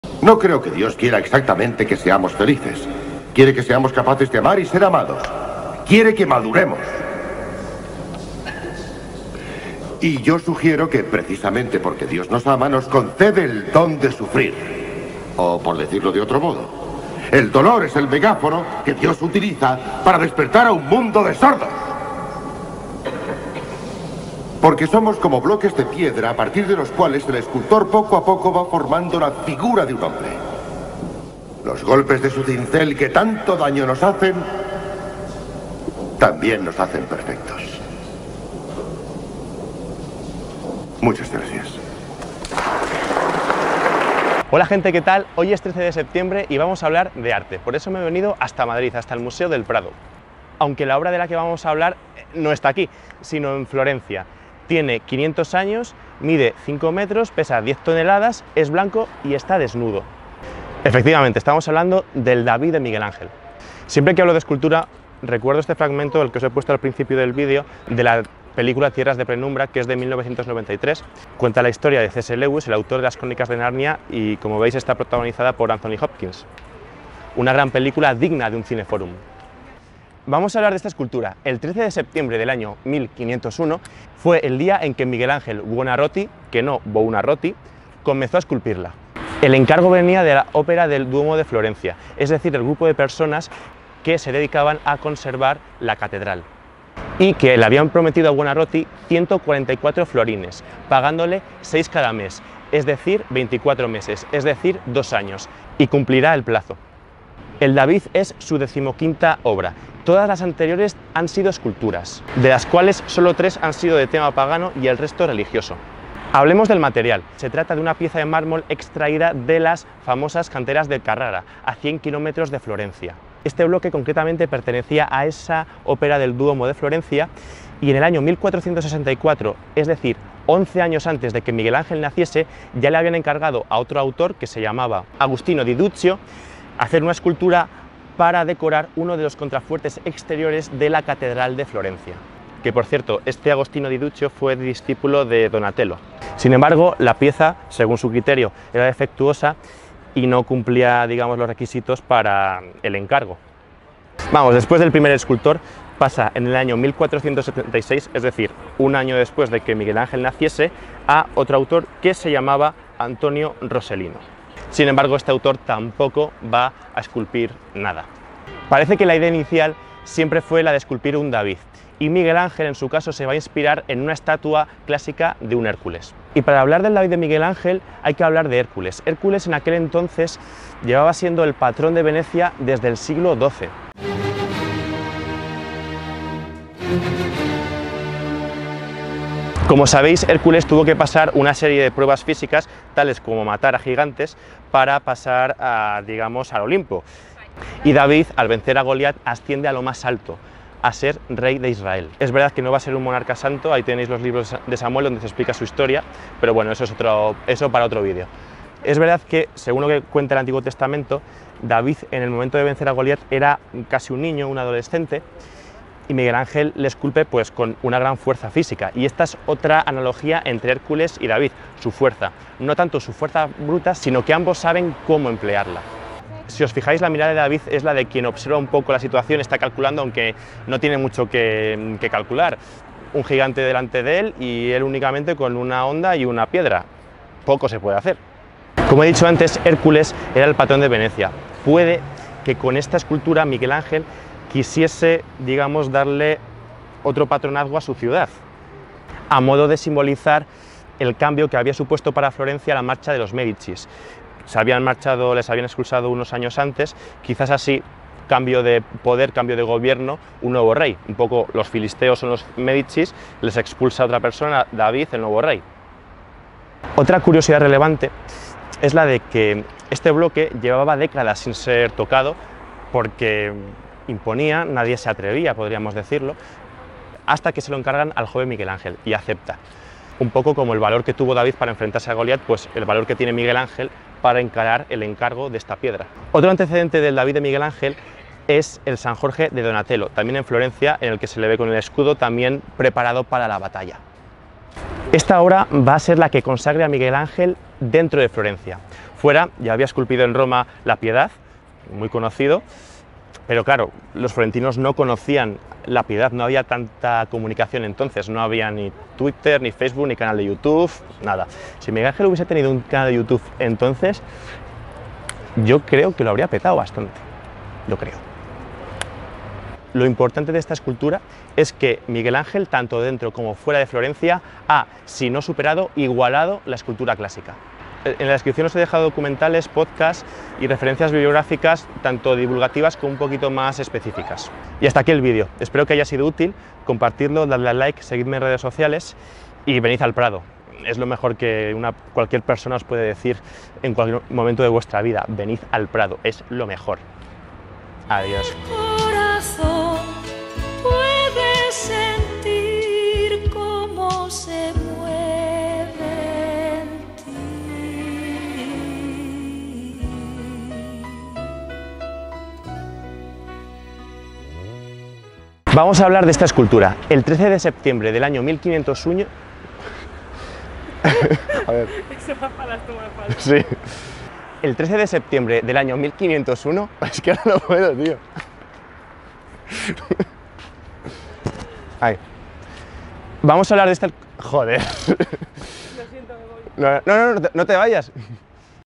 La escultura más perfecta del sistema 🌞 EFEMERIDE4 | 13.sept.1501
¡El David de Miguel Ángel como nunca te lo habían contado: desde el Museo del Prado!

SINOPSIS.
El 13 de septiembre de 1501, Miguel Ángel Buonarroti comienza a esculpir la obra que le consagrará como escultor. Es verdad que en sus más de sesenta años de oficio, ejercerá de pintor y arquitecto, pero él siempre se denominó escultor. Y esta obra, la 15ª de su carrera, habla por sí sola.

BIBLIOGRAFÍA.
Giorgio Vasari. Las vidas de los más excelentes arquitectos, pintores y escultores italianos desde Cimabue a nuestros tiempos. Ed. Tecnos/ Alianza, 2004.
Jesús Hernández Perera. El Cinquecento y el manierismo en Italia. Ed. Historia 16, 1989.
Charles de Tolnay. Miguel Ángel escultor, pintor y arquitecto. Ed. Alianza Forma, 1999.
Eugenio Battesti. Miguel Ángel, escultor. Ed. Atlantis, 1989.

ENLACES.

MÚSICA.

AGRADECIMIENTOS.
T. Gutierrez.
By Gonzalo H., graduando en Historia.